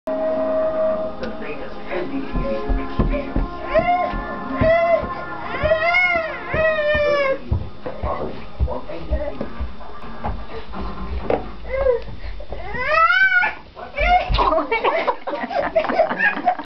Google Slides
This is in Birmingham ... Google doesn't know how to walk down the stair - he just slides ...